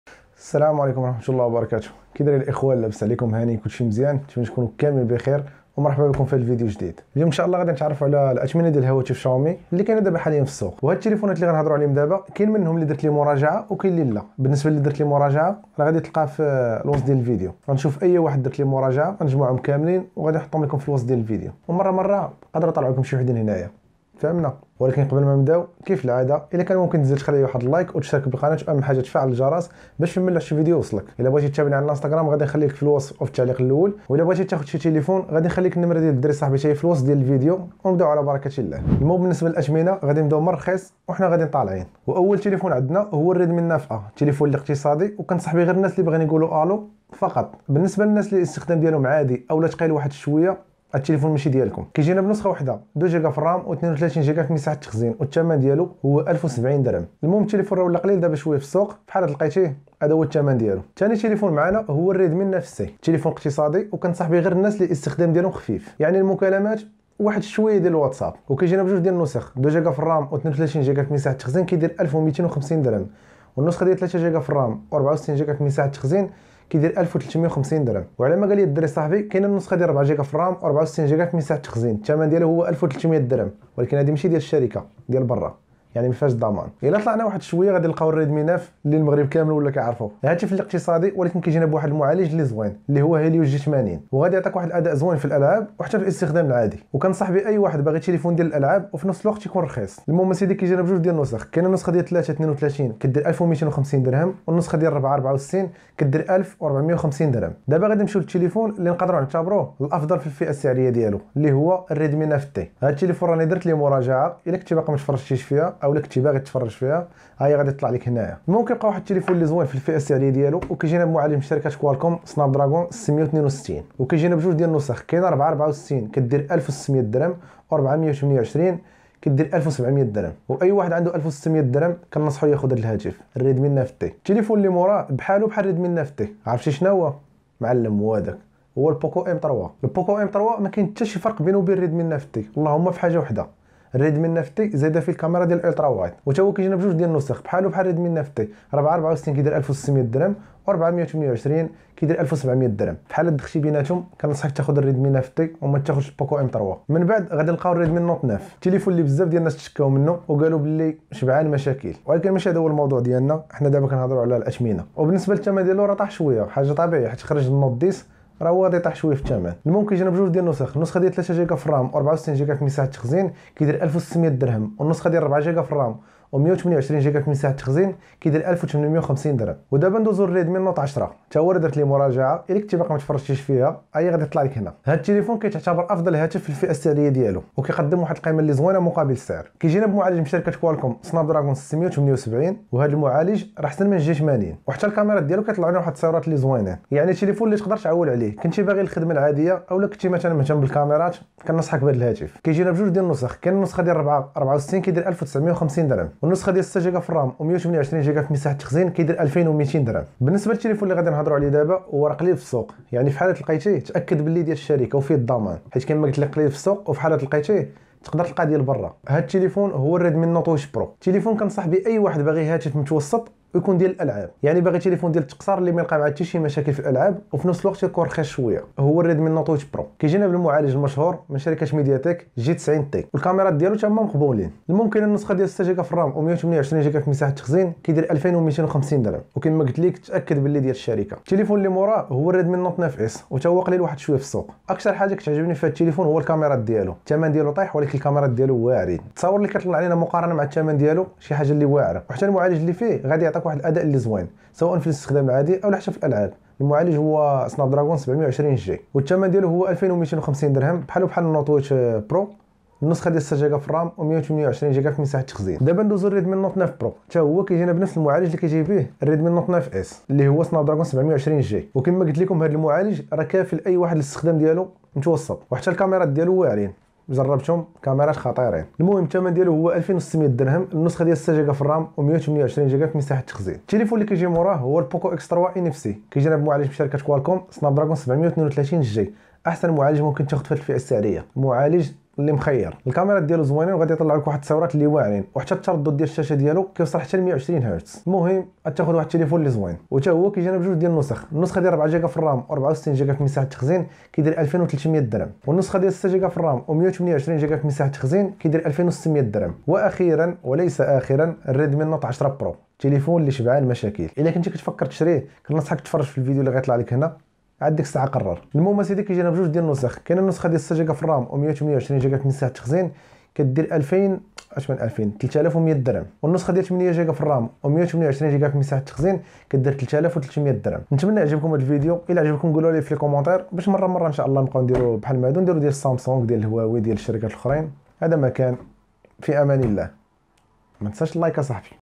Xiaomi | اثمنة هواتف شاومي في المغرب
في هذا الفيديو غادي نتعرو على الاثمنة ديال هواتف شاومي لي كينا دابا حاليا فالسوق المغربية.

رقم الصديق بيشالا :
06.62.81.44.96